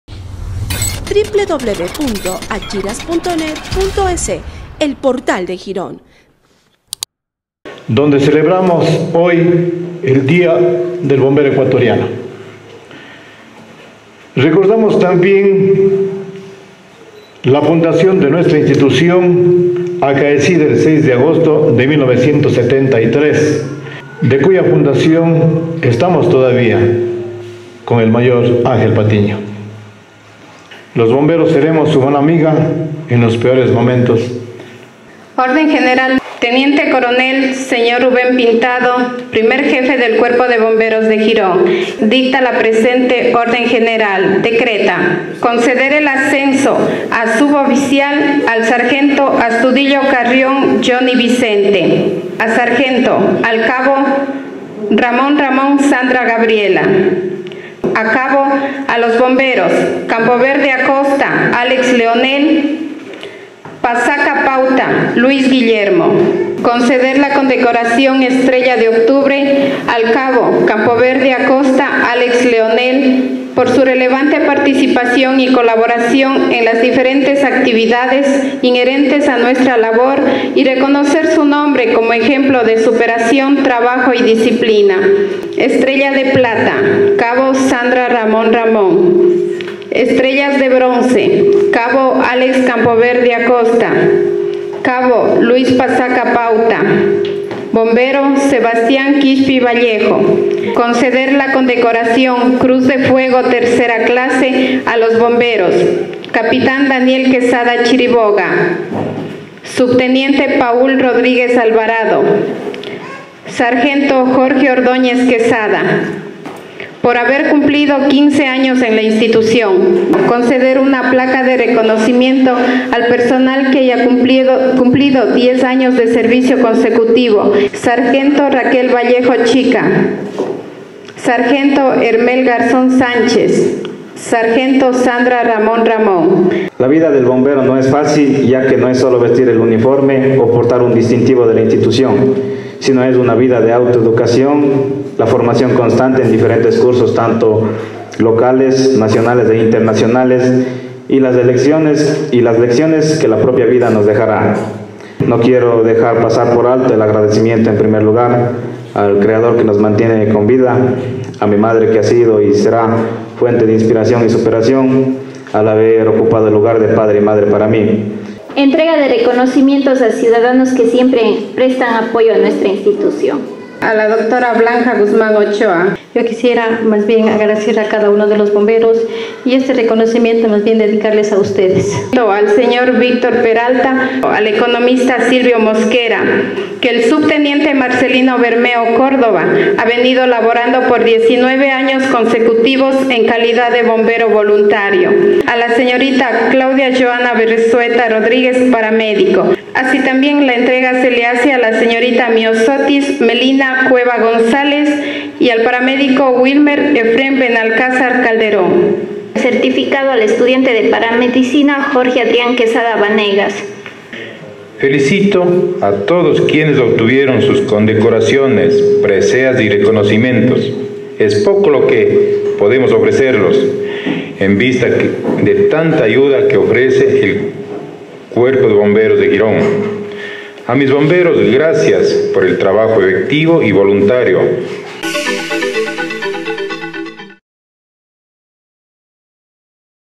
Bomberos celebraron su día clásico con desfile y sesión solemne
Cada 10 de octubre en Ecuador se celebra el día del Bombero, en Girón la institución desarrolló su habitual desfile por la calles céntricas de la ciudad y posterior sesión solemne a la cual asistieron autoridades y ciudadanía.
El jefe de los voluntarios recordó el motivo de la creación de la institución de socorro. Parte del programa se destinó al acto de ascenso y reconocimientos al personal que ha cumplido con los requisitos y que han destacado en el cumplimiento de su misión; de la misma forma se entregaron reconocimientos a ciudadanos que apoyan a la entidad de socorro.